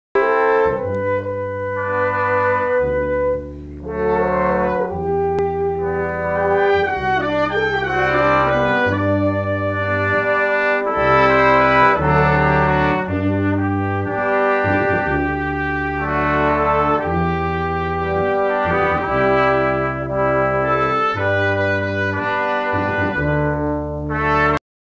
Howard County Concert Orchestra moment of music
This is a moment from the concert held on October 20th, 2013 where we hosted a special afternoon at Artistic Director, Ronald Mutchnik's home.